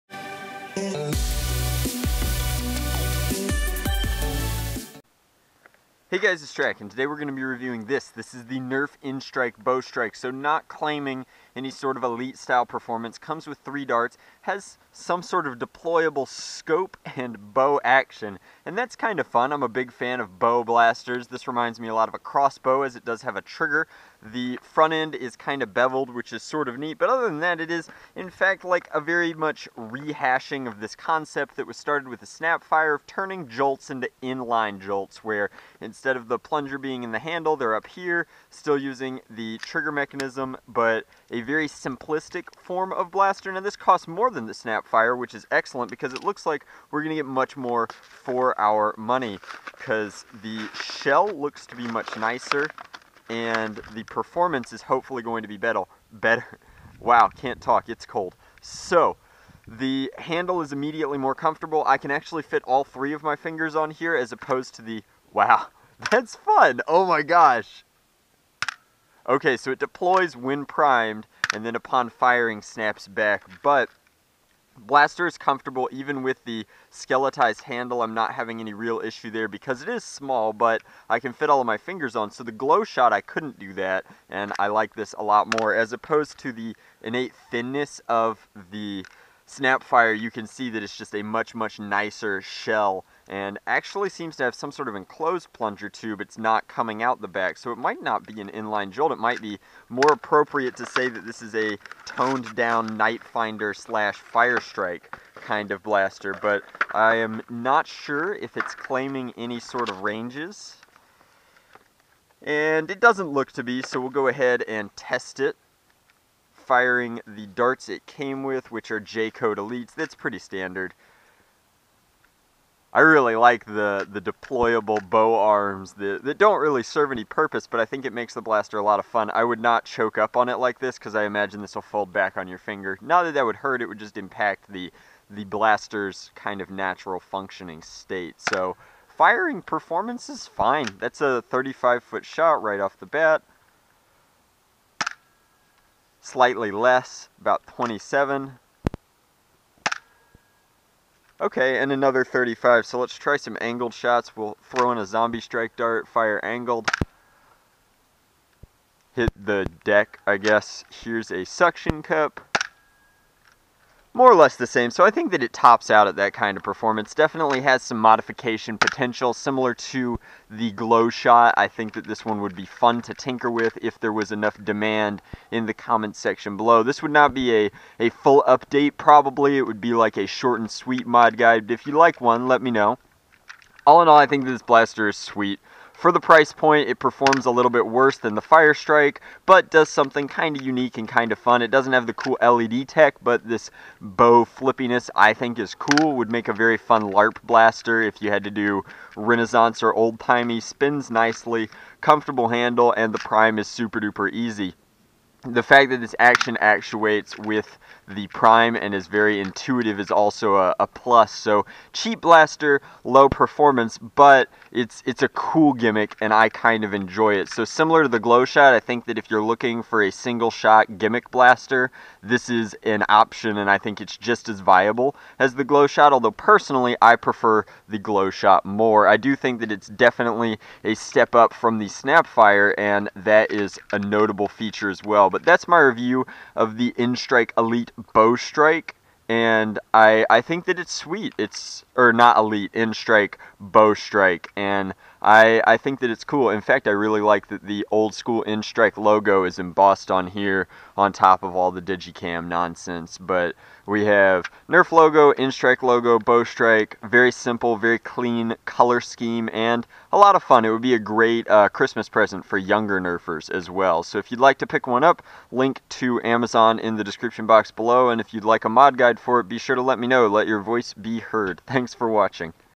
Honest Review: Nerf Elite BowStrike (Nerf's Smallest "Crossbow" Yet)
On Amazon
A really fun little blaster, the bowstrike integrates some lightweight bow arms and a reticle into its priming action.  This makes it a fun gimmicky sidearm that fits into the proper price point.